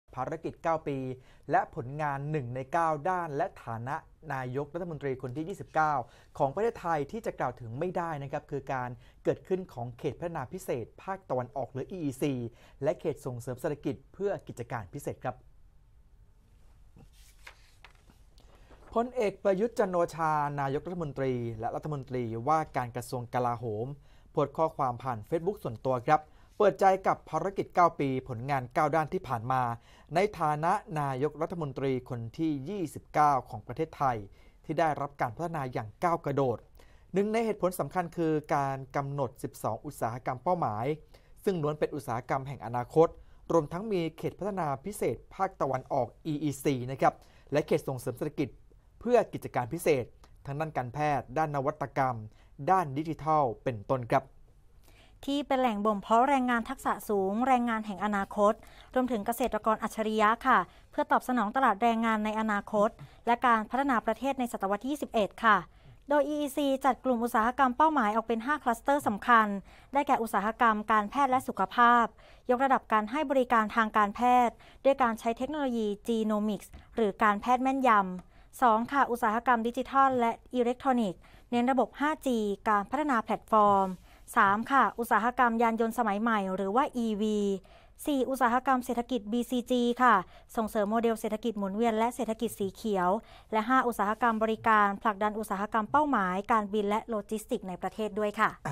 ภารกิจ 9 ปี และผลงาน 1 ใน 9 ด้านในฐานะนายกรัฐมนตรี คือการเกิดขึ้นของเขตพัฒนาพิเศษภาคตะวันออก (EEC)
ภารกิจ 9 ปี และผลงาน 1 ใน 9 ด้านในฐานะนายกรัฐมนตรีคนที่ 29 ของประเทศไทยที่จะกล่าวถึงไม่ได้ คือการเกิดขึ้นของเขตพัฒนาพิเศษภาคตะวันออก (EEC) และเขตส่งเสริมเศรษฐกิจเพื่อกิจการพิเศษ